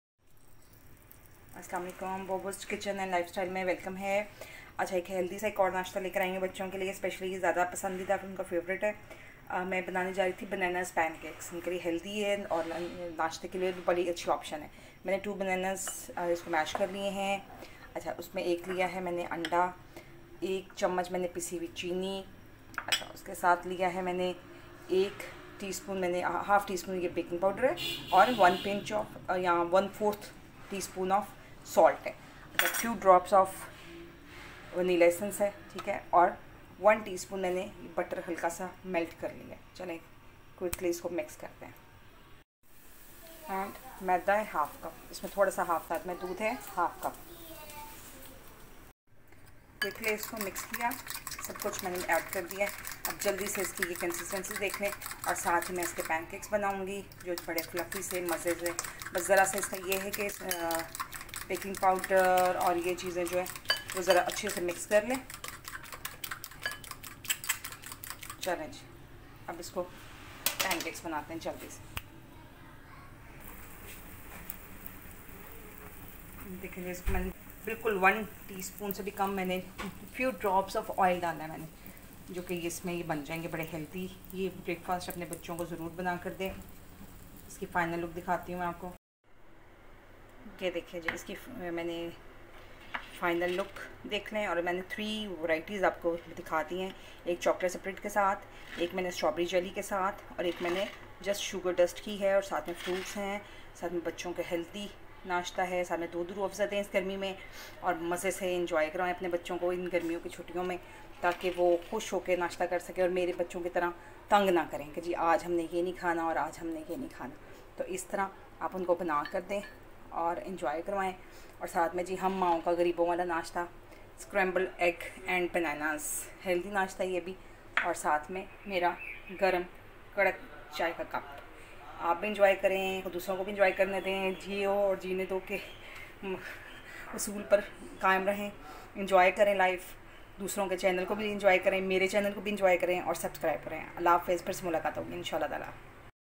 Quick n easy Banana Pan Cakes🥞
A healthy breakfast option, quick n easy, delicious n yummy, favourite thing of kids.